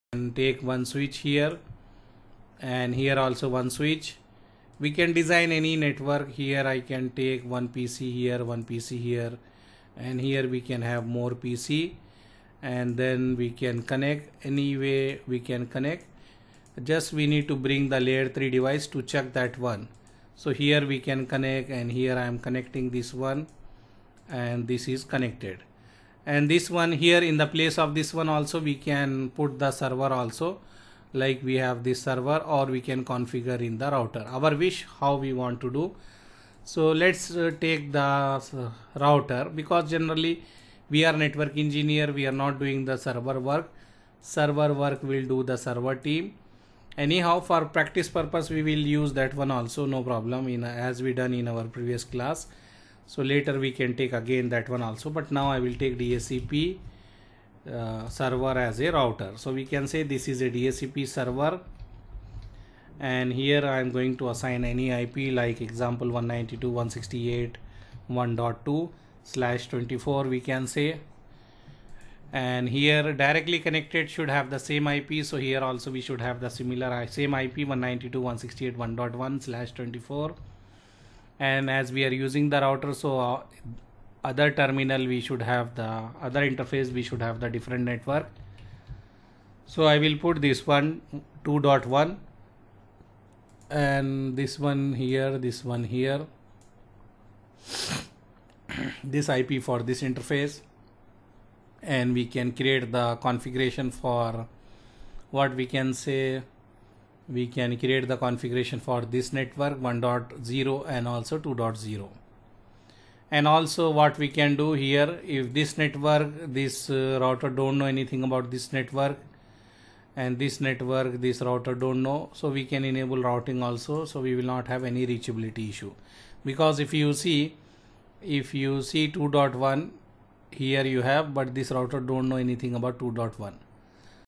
Basic design in CISCO Packet Tracer for DHCP | Basic Lab Topology Overview
Hello Everyone,

In this video we are going to discuss Basic design in CISCO Packet Tracer for DHCP.
Thanks.

About us:
Thanks